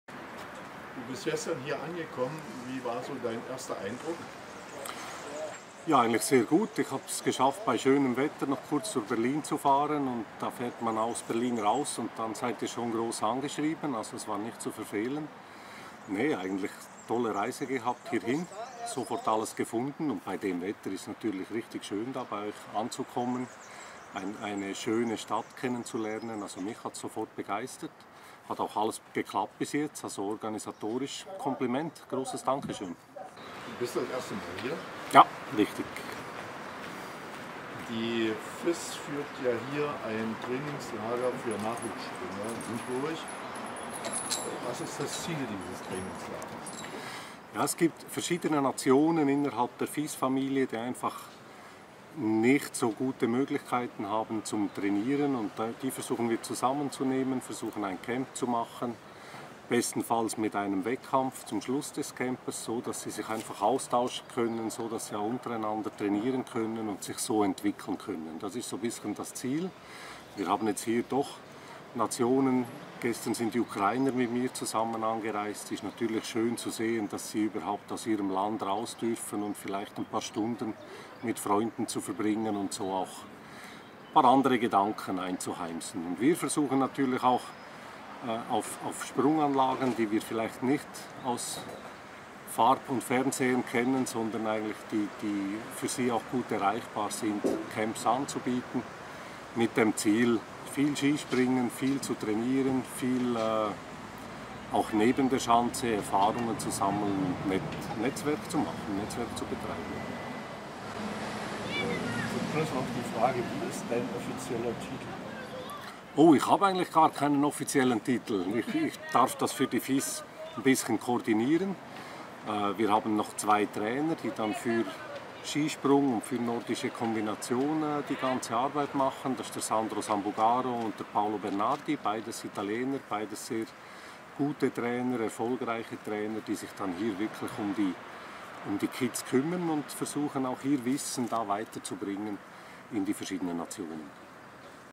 Interview mit Berni Schödler, FIS Koordinator, in der Sparkassen-Skiarena Bad Freienwalde
FIS Trainingslager endet am Sonnabend mit internationalem Wettkampf
Europäische Nachwuchssportler und Sportlerinnen von der Fédération Internationale de Ski (FIS) mit Sitz in Zürich nach Bad Freienwalde eingeladen
Die FIS hat Bad Freienwalde nicht vergessen. Nachdem vor einigen Jahren bereits die jährliche Tagung des Skiverbandes in der Kurstadt stattfand, sich inzwischen die Sparkassen-Ski-Arena zu einer der besten internationalen Schanzenarenen  für das Nachwuchsspringen entwickelt hat, findet in dieser Woche ein internationales Trainingslager der neun- bis 18jährigen Springer und Nordisch Kombinierten aus Georgien, Ungarn, Lettland, Rumänien, der Slowakei und der Ukraine statt. Geleitet wird das Training vom FIS COC Coordinator Ski Jumping Berni Schödler. Schödler war Schweizer Nationaltrainer und führte Simon Ammann 2002 zum zweifachen Olympiasieg. Ziel des Trainingslagers sei es, vor allem den Nationen zu helfen, denen es finanziell schwerfällt, den Nachwuchs im Skispringen und in der Nordischen Kombination zu fördern. Und natürlich sollen die teilnehmenden Nationen auch einen internationalen Vergleich haben. Besonders freue er sich, sagte Berni Schödler, dass es gelungen sei, auch ukrainische Sportler dabei zu haben.
Trainiert werden die Sportler und Sportlerinnen von den beiden Italienern Sandro Sambugaro (ehemaliger italienischer Skispringer, unter anderem Vierschanzentournee) und Paolo Bernardi (ehemaliger Trainer der USA – Frauen – Nationalmannschaft und Italiens Männermannschaft) und natürlich von den nationalen Trainern. Auch unsere Bad Freienwalder Springer und Springerinnen werden sich so manchen Kniff abgucken.
Neben den sportlichen Aktivitäten gibt es natürlich auch ein Freizeitprogramm bei dem es am Donnerstag unter anderem in den Bundestag gehen wird.
Wer die jungen Athleten beim Training beobachten will, hat jeden Tag dazu die Möglichkeit im Papengrund. Höhepunkt wird am Sonnabend der internationale FIS – Wettkampf im Spezialsprunglauf und in der Nordischen Kombination sein. Ab 9.00 Uhr messen sich dann die Sportler und Sportlerinnen aus Georgien, Ungarn, Lettland, Rumänien, der Slowakei und der Ukraine mit den deutschen und polnischen Rivalen im sportlichen Wettstreit um den Sparkassenpokal. Mit dabei sein werden auch alle Bad Freienwalder Asse um Max Unglaube, Moritz Terei, Alvine Holz, Mila Twarok und Florian Fechner. Gefördert wird das Projekt auch vom Brandenburgischen Ministerium für Bildung, Jugend und Sport und von der Europäischen Union.